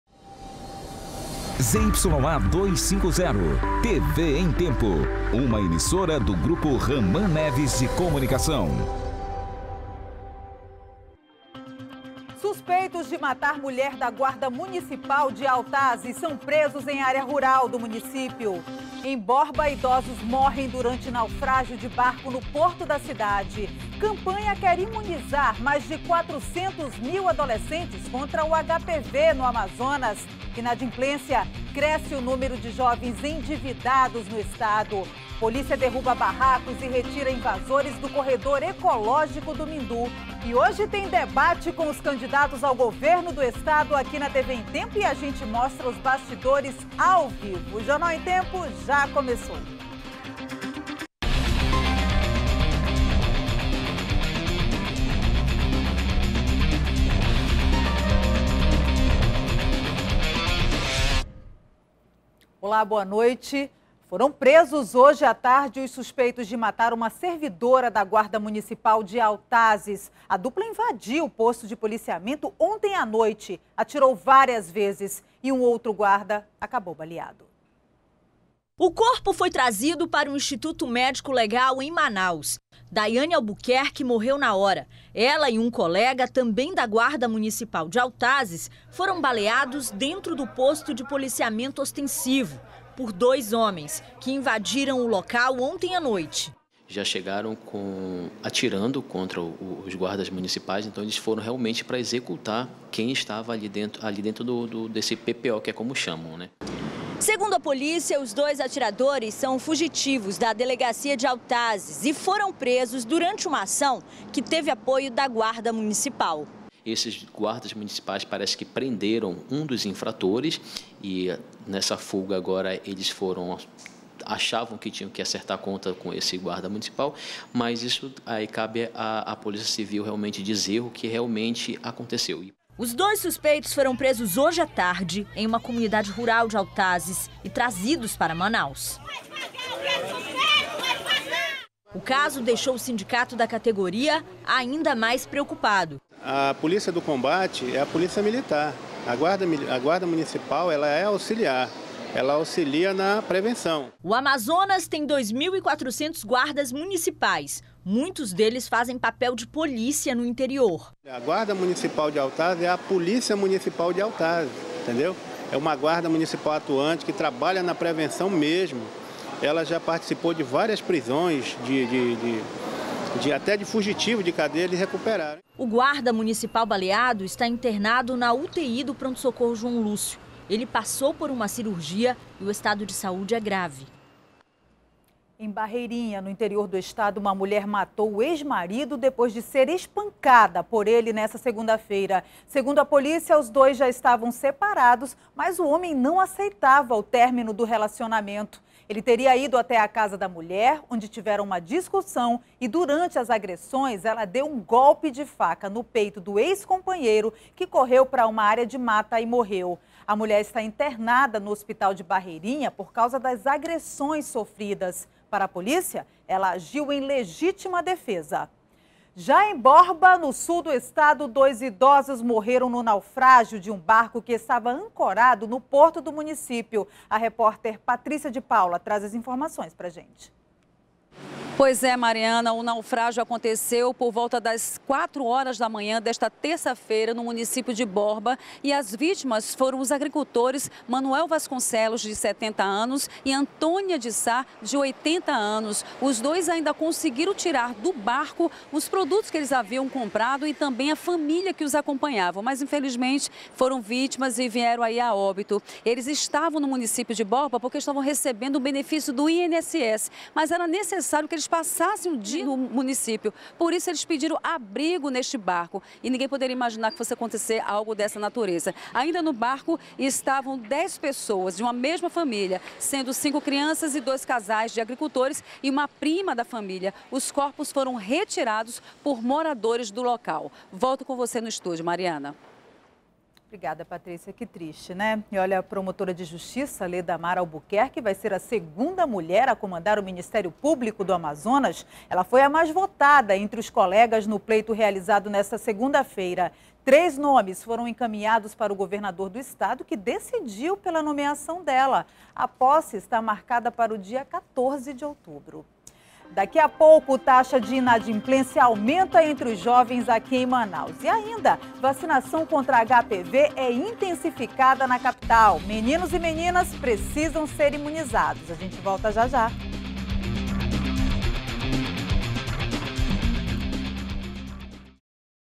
Suspeitos de matar uma servidora da guarda municipal de Autazes são presos
01º BLOCO JORNAL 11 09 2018 - Suspeitos de matar uma servidora da guarda municipal de Autazes são presos